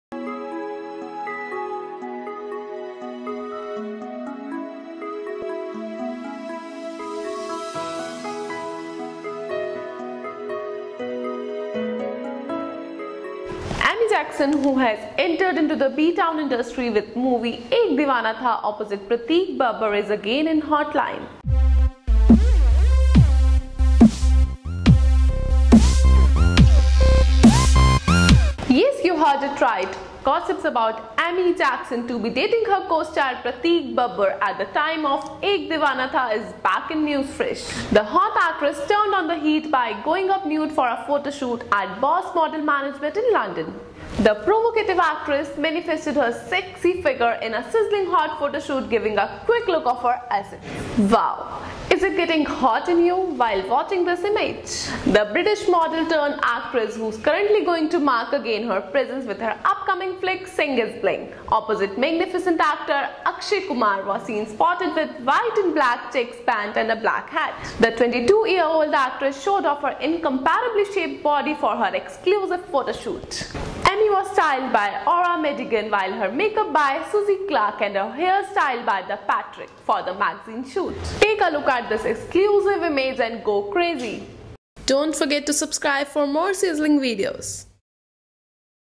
Amy Jackson NUDE Photos Leaked | Vscoop
Hot actress amy jackson turned on the heat by going up nude for a photoshoot at boss model management in London